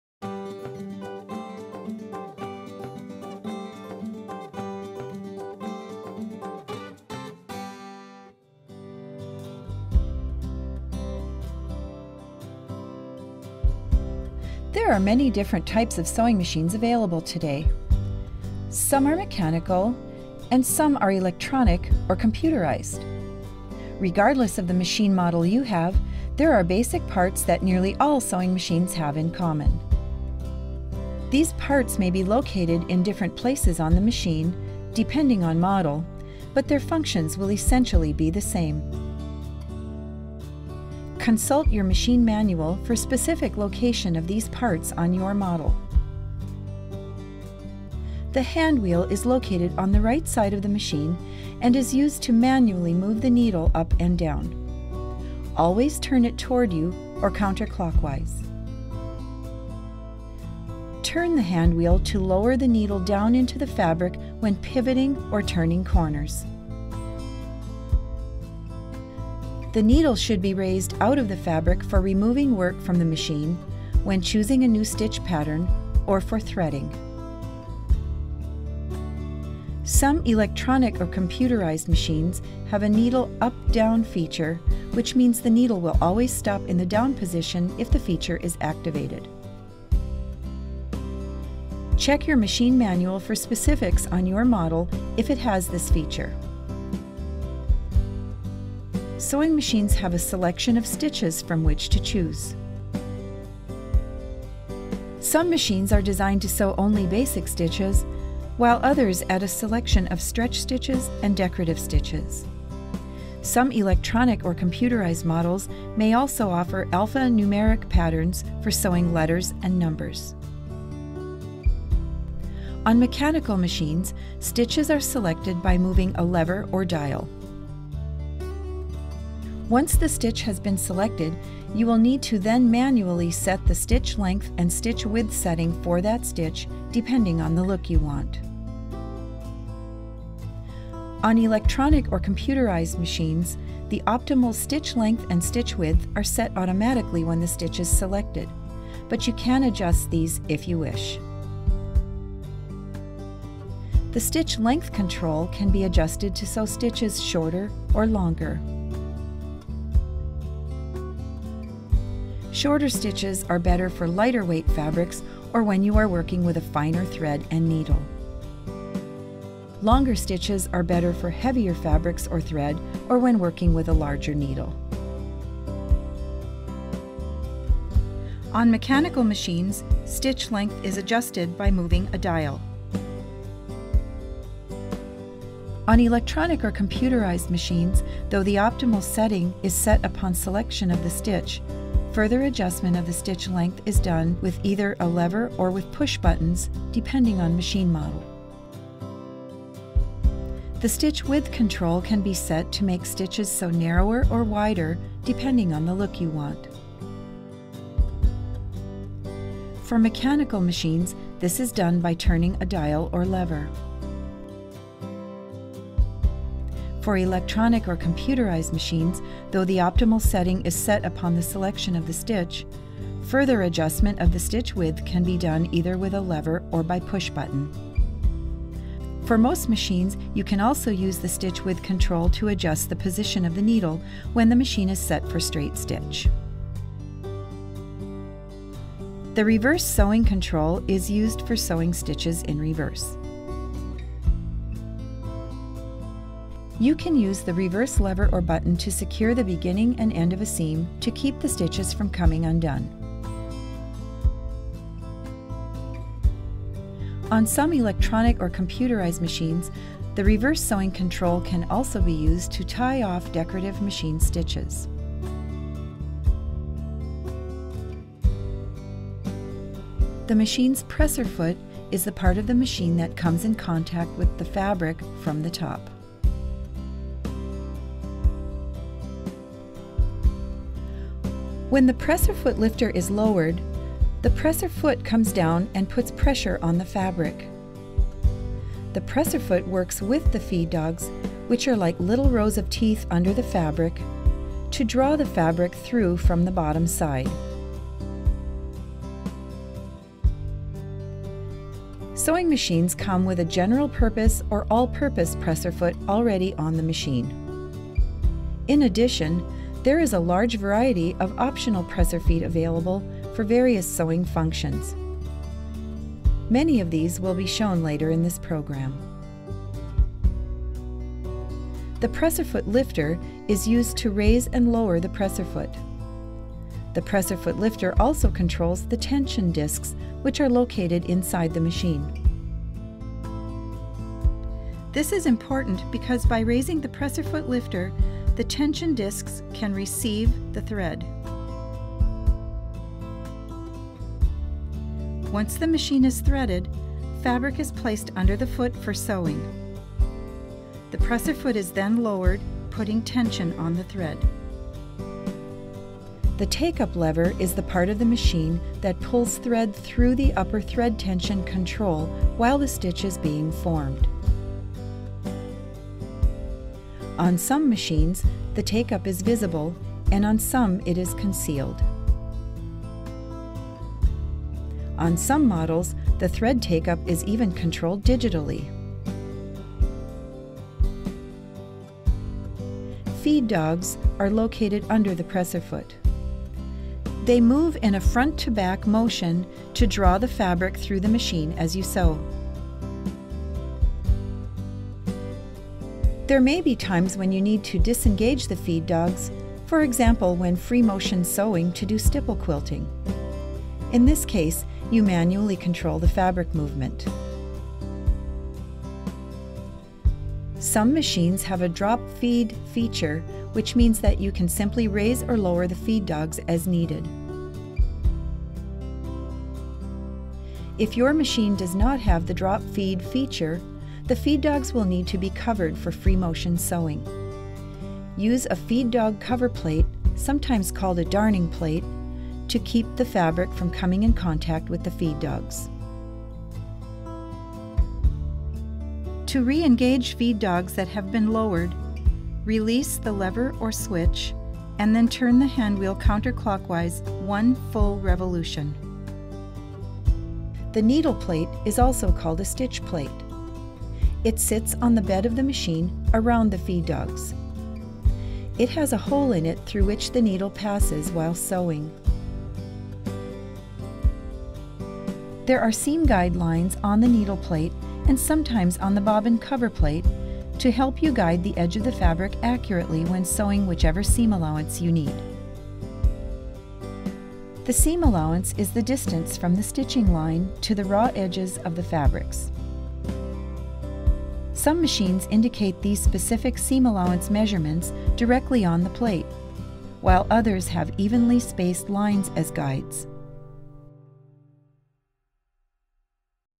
SINGER® Sewing Machine Basics
Learn the basics about your machine and how to get started sewing.